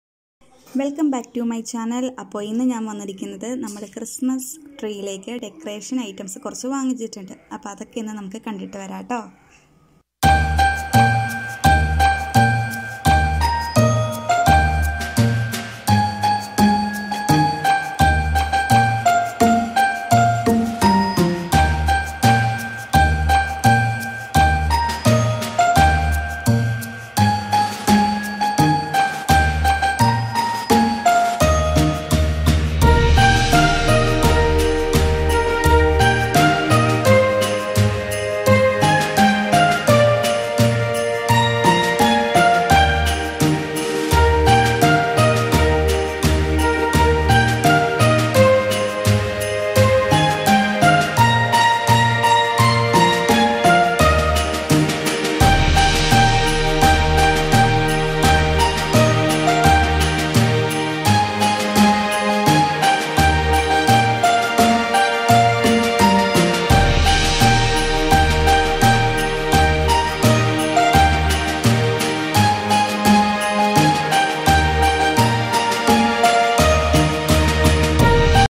ഞങ്ങൾ വാങ്ങിയ XMAS Tree decoration Items..
Christmas tree decorate ചെയ്യാന്‍ വാങ്ങിയ items ആണ് ഈ വീഡിയോയില്‍ ഉള്ളത്..